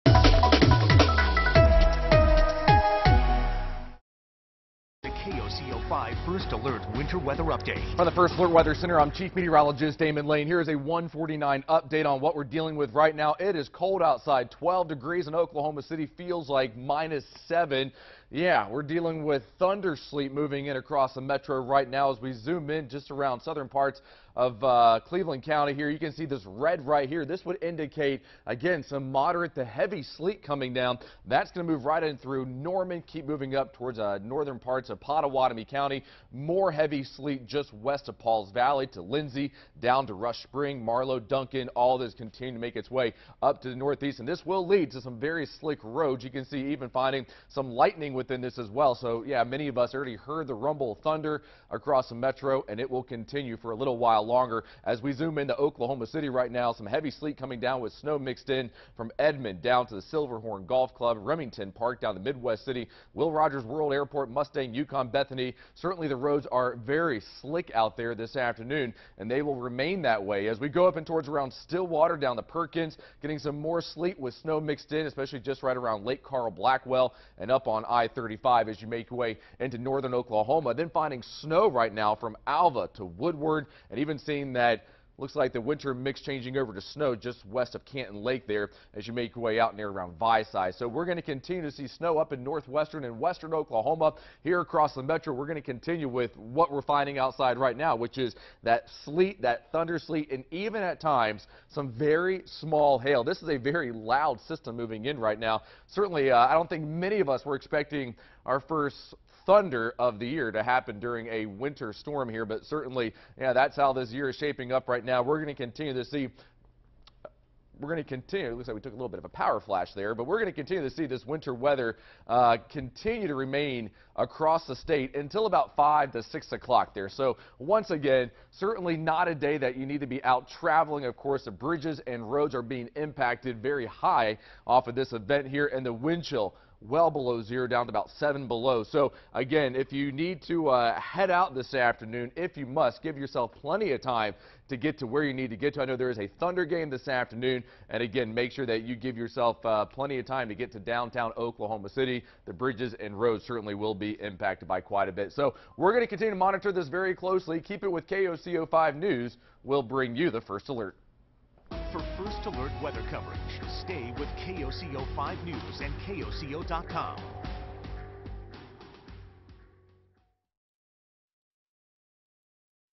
Forecast: Winter storm continues in Oklahoma
A winter storm continues to impact the Oklahoma City metro and areas around the metro. Snow, sleet and ice is expected to fall in the metro until about 9 p.m.